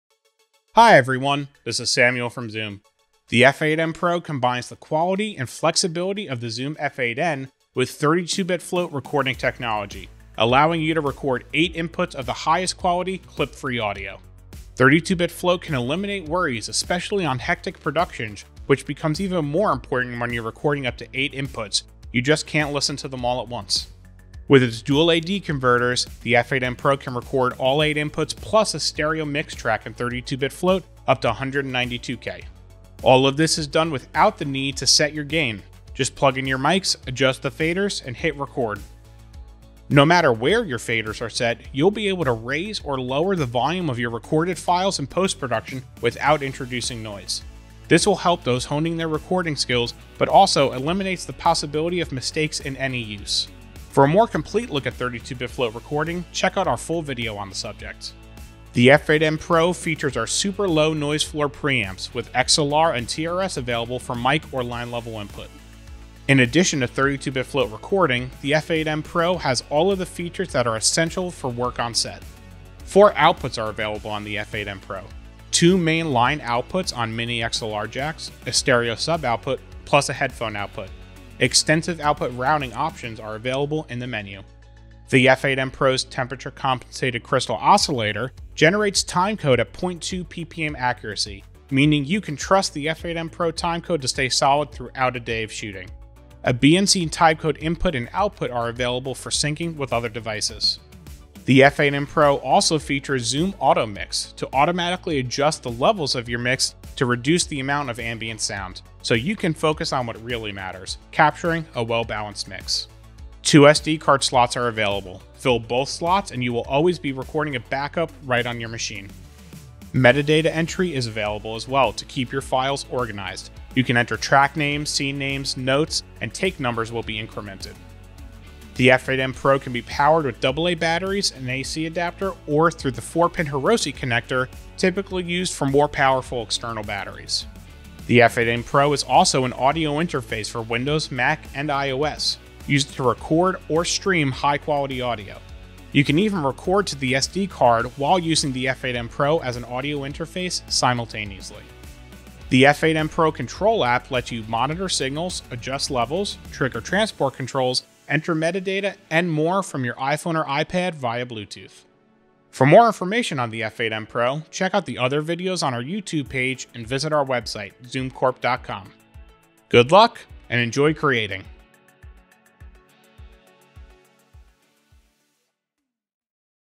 Meet the Zoom F8n Pro Field Recorder
Zoom Product Specialist, Samuel Greene, showcases our newest flagship multi-track field recorder, the F8n Pro. Record on 8-inputs simultaneously with 32-bit float technology for a full dynamic range and clip-free audio. Take control of your hectic and complex productions, eliminate the need to adjust gain and monitor each track, and use the highest quality sound that will enable you to make magic in post.